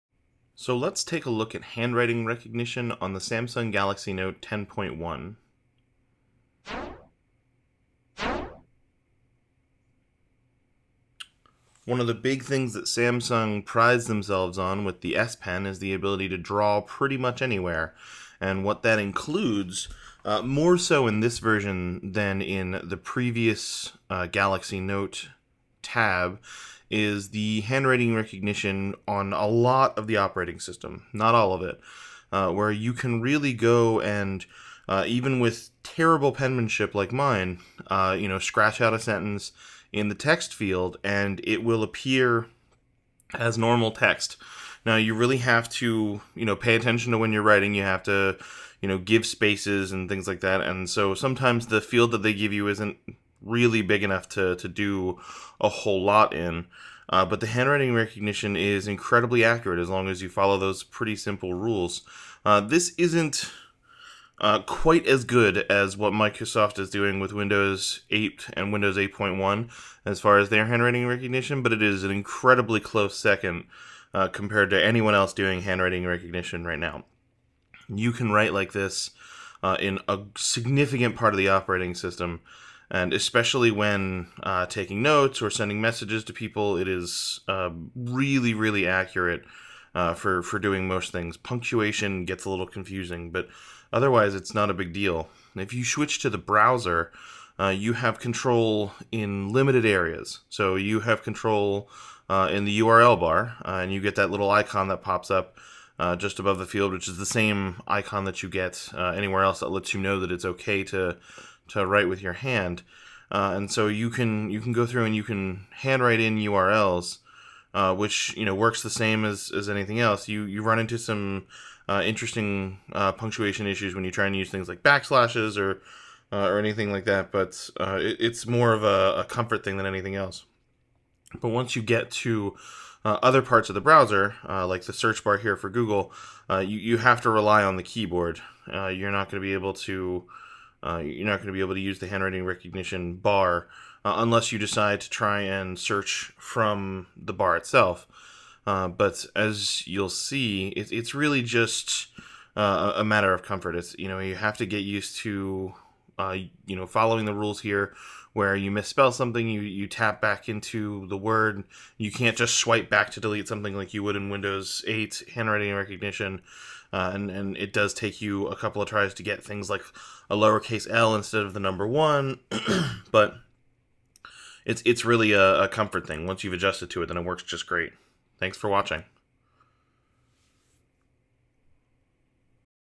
Handwriting recognition on the Note 10.1 - 2014 edition
The S Pen on the Galaxy Note line has always been a big differentiator for Samsung,  and in every version update of this hardware line there has been major improvements.  One of the most significant for this latest hardware release is the handwriting recognition software, which is easily some of the best being used today.